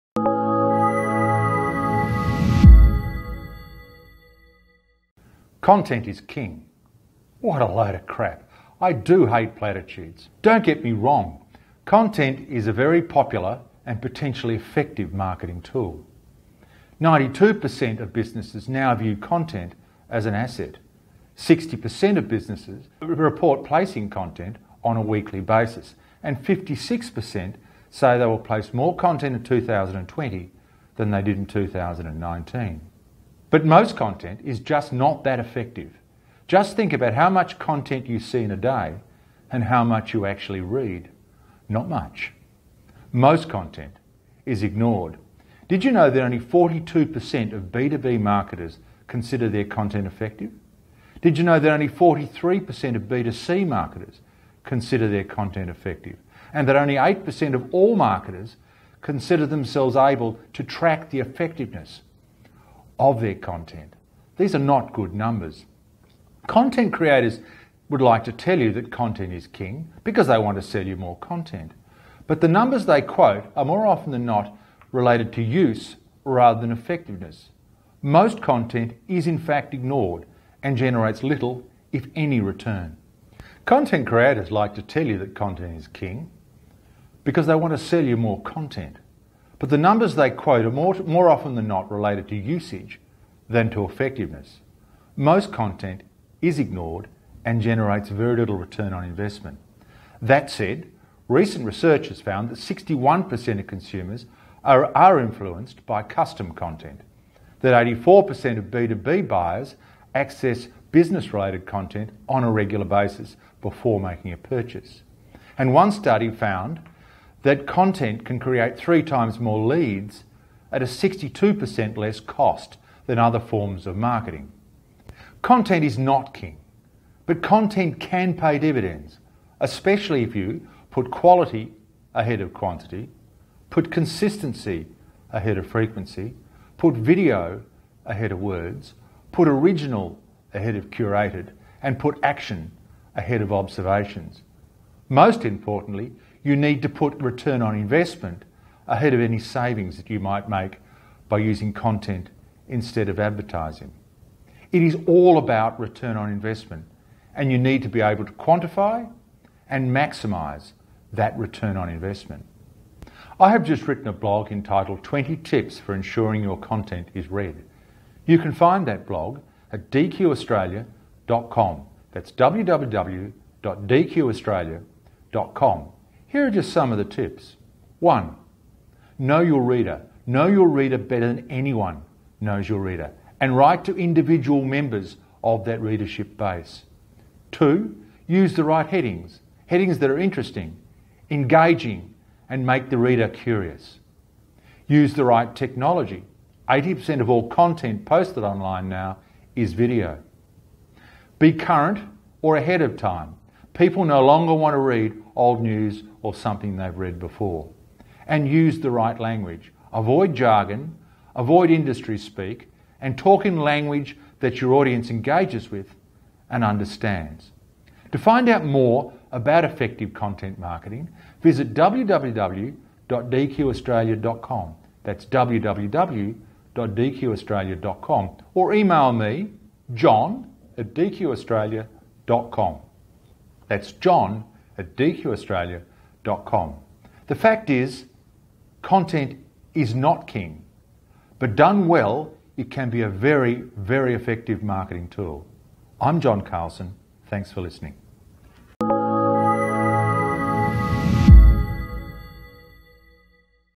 Decoding the theory behind the Content is King" by D. John Carlson at DQAustralia
Content is king and will always be!
But how to ensure your content is reached, read and consumed with the targeted audience?
In this video, we will go through some facts and data about the importance of content in business marketing. Also, by the end of the video, you will get to know the 20 tips to ensure your content is read.

Do you know 92% of businesses view content as an asset and 60% of businesses post content on a regular basis!
No doubt, content is a very popular and potentially effective marketing tool. But most of the content is not just effective. On top of that most of the content is just ignored or generates little return.

So, here are some tips to make content effective and helpful:
1) Put quality ahead of quantity
2) Put consistency ahead of frequency
3) Put video ahead of words
4) Put original ahead of curated
5) Put action ahead of observations
6) Most importantly, put ROI ahead of saving